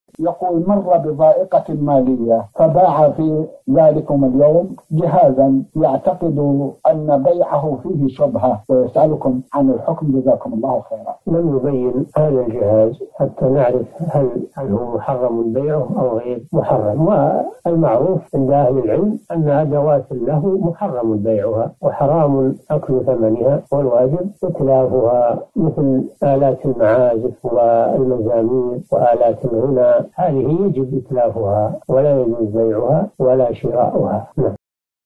مر بضائقة مالية فباع في ذلكم اليوم جهازا يعتقد أن بيعه فيه شبهة ويسألكم عن الحكم؟ الشيخ صالح الفوزان
نص السؤال: مرَّ بضائقةٍ مالية، فباع في ذلكم اليوم جهازًا يعتقدُ أن بيعه فيه شبهة، ويسألكم عن الحكم؟
السيرة الذاتية :
للاشتراك في قناة الشيخ صالح الفوزان

رئيس الفريق
لمشروع كبار العلماء